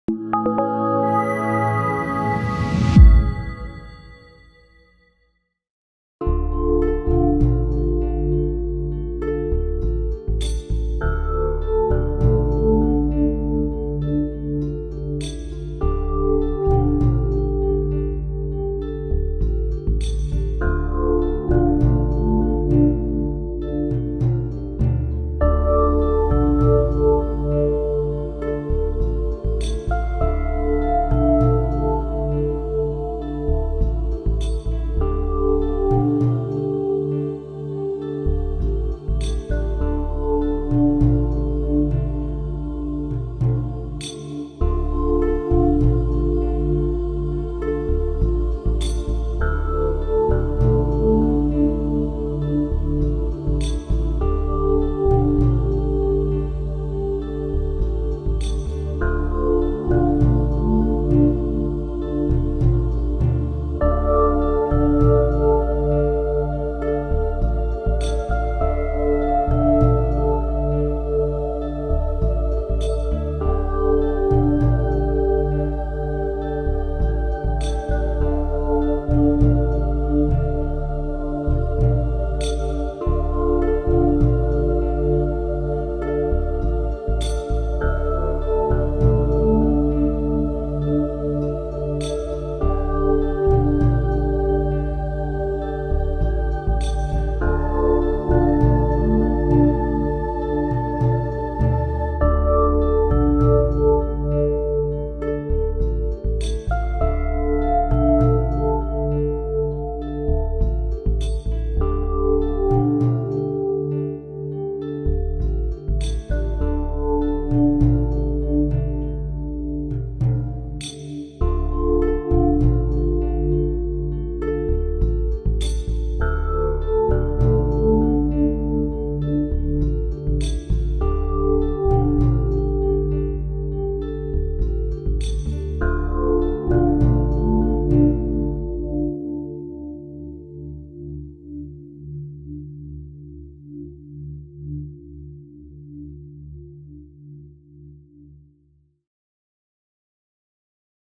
“曼巴精神”永存！直升机坠机原因推测
NTSB发布科比直升机坠机的调查视频以及失事前的通话录音，根据此我们可以推测出一些空难的可能性原因